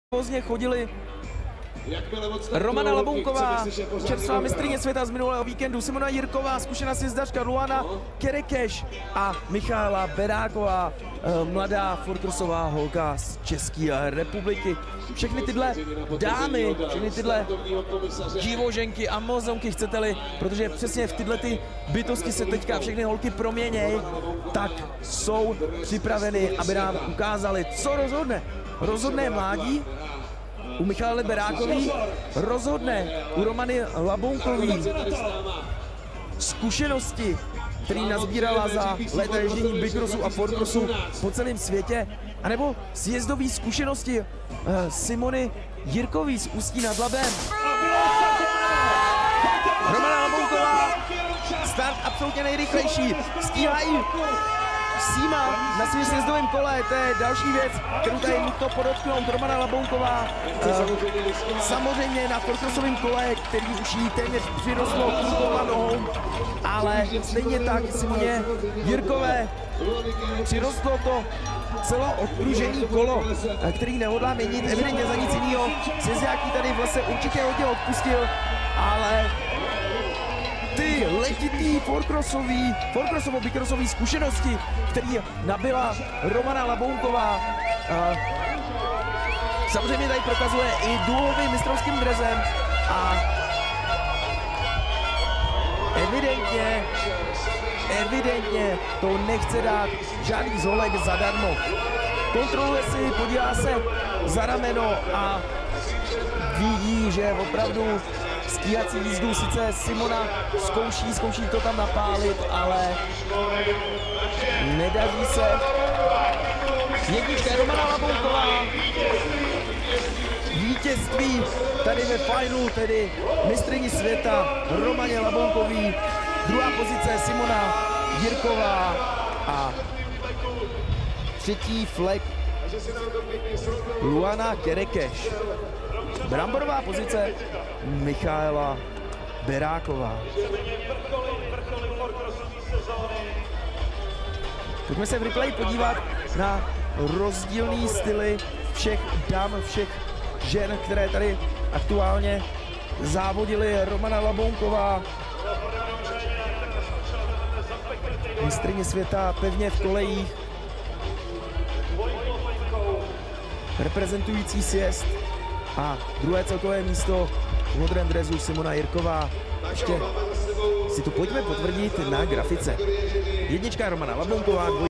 JBC 4X Revelations 2018 - final WOMEN
1. Romana Labounkova
2. Simona Jirkova
3. Luana Cherches
4. Michaela Berakova